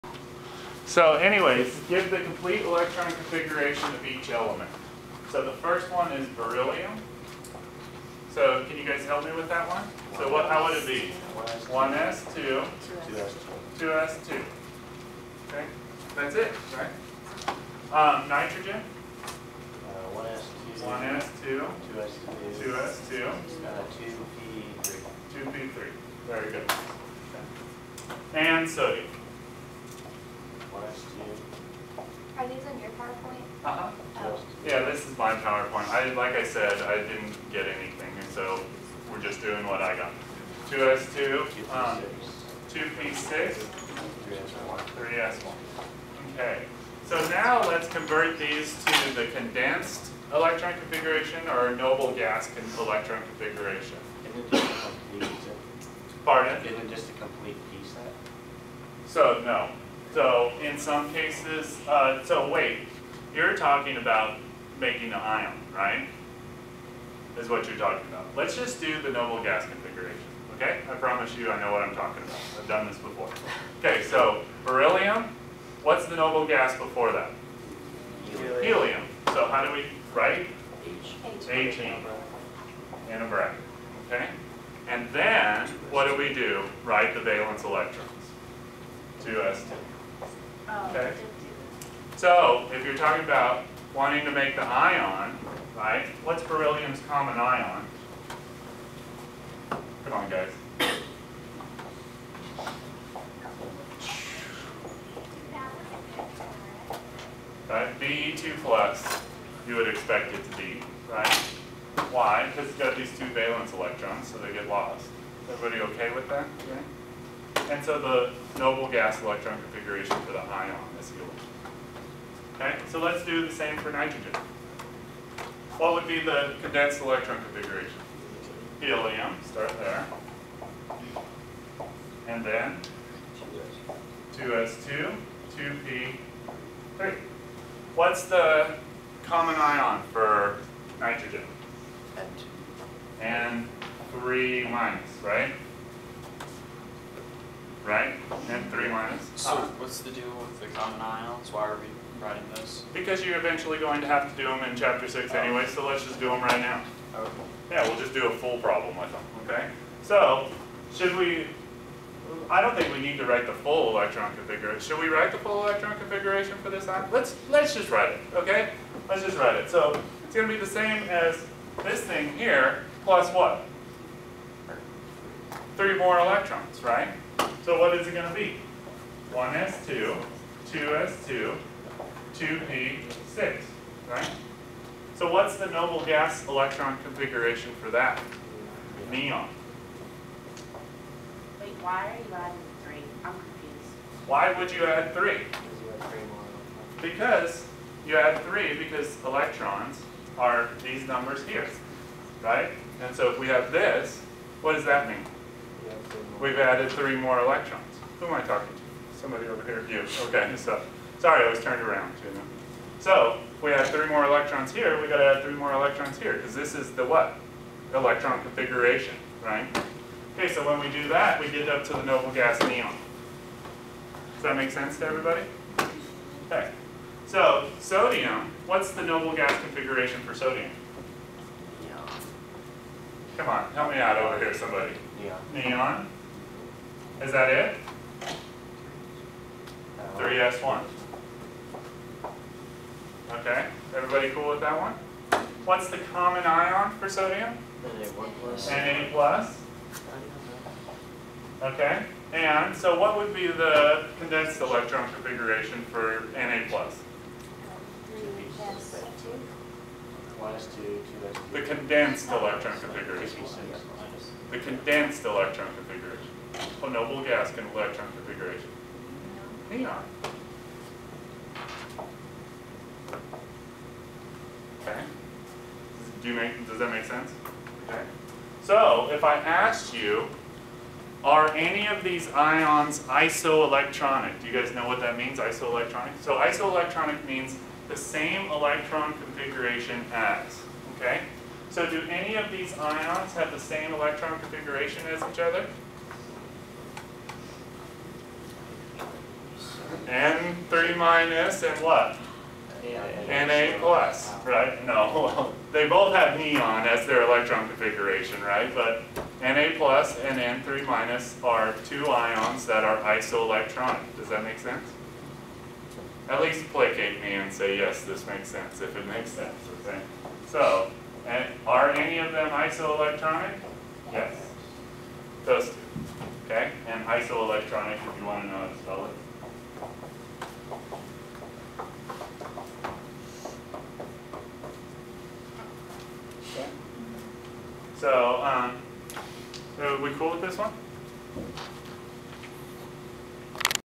Isoelectronic or not - Electron Configurations of Ions 003
For each of the following elements (Be, N, Na):
a) Write the complete electron configuration.
b) Write the condensed electron configuration.
c) Predict the common ion of the atom.
d) Write the condensed electron configuration of the ions.
e) Are any of these ions isoelectronic?